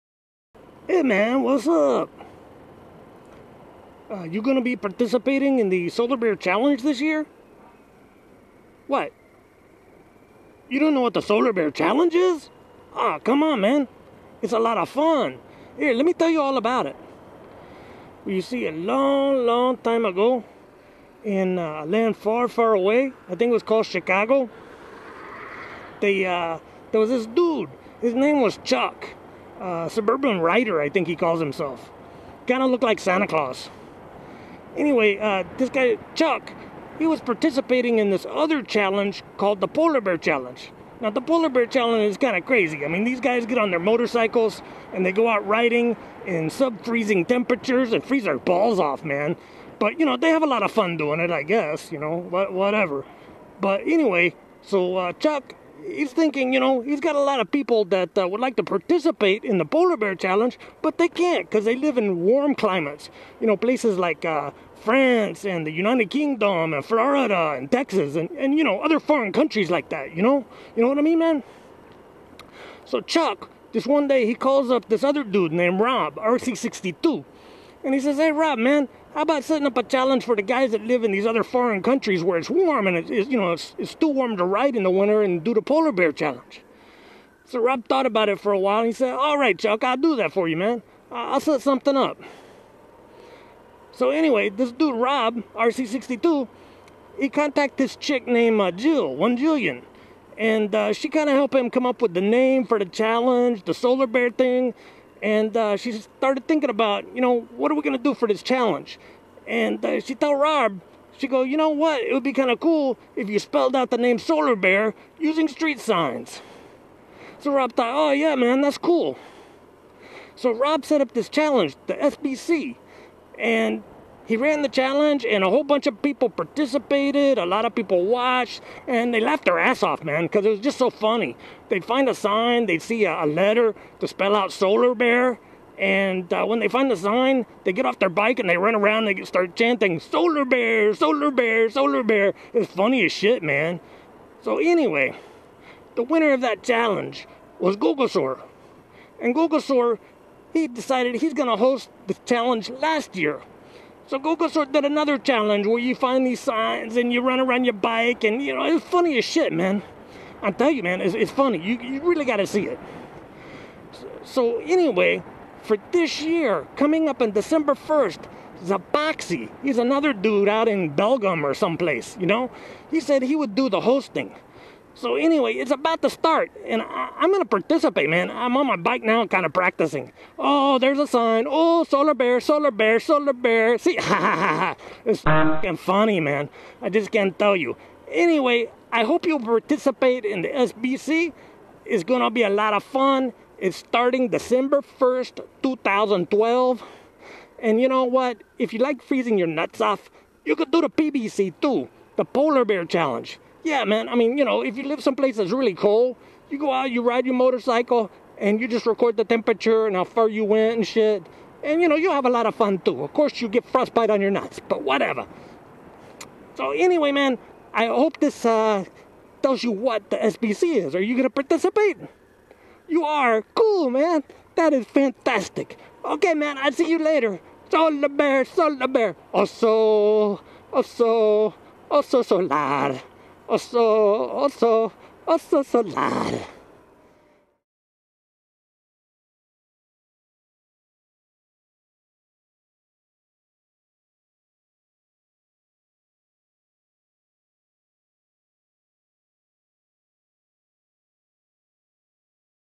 Solar Bear Challenge III Promotion by R'Cheech'62
My good friend R'Cheech'62 created this promotional video talking about the Solar Bear Challenge III (SBC III).  :P